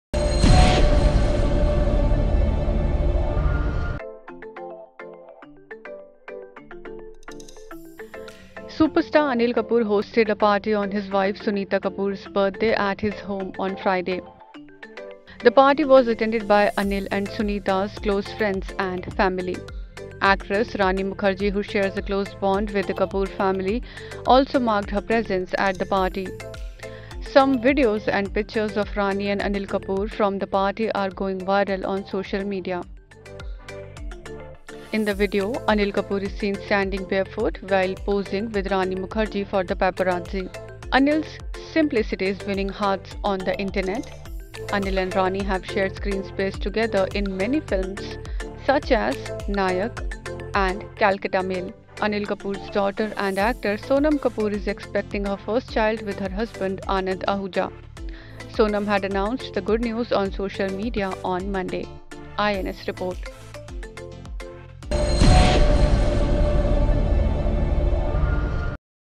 When Anil Kapoor forget to wear footwear
Superstar Anil Kapoor hosted a party for his wife Sunita Kapoor on Friday.